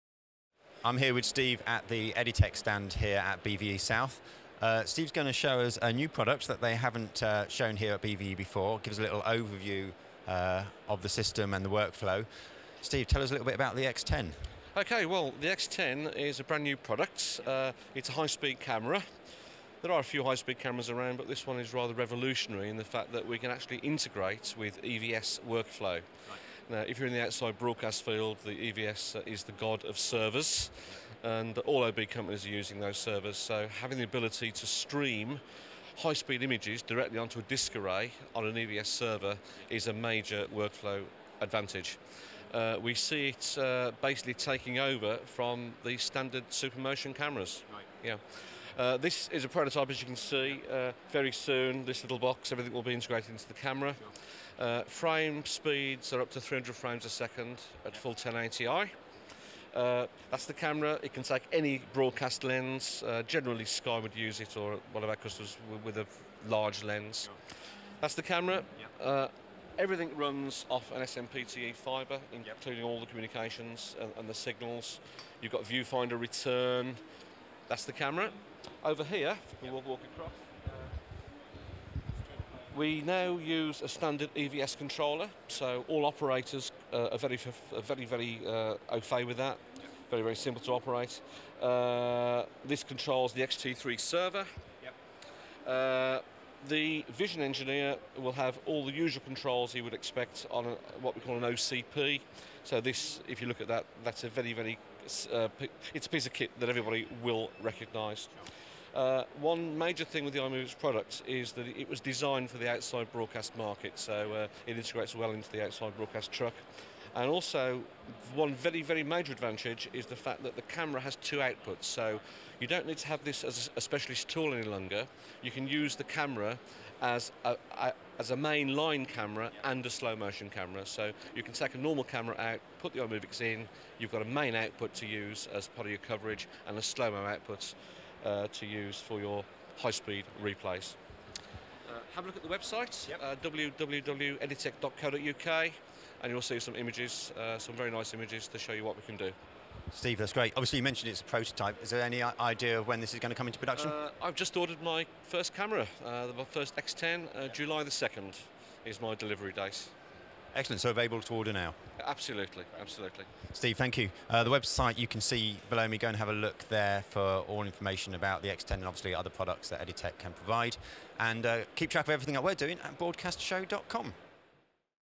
Editec at BVE 2012
Steve from Editec shows a new product, the X10 high speed camera, which intergrates with EVS workflow.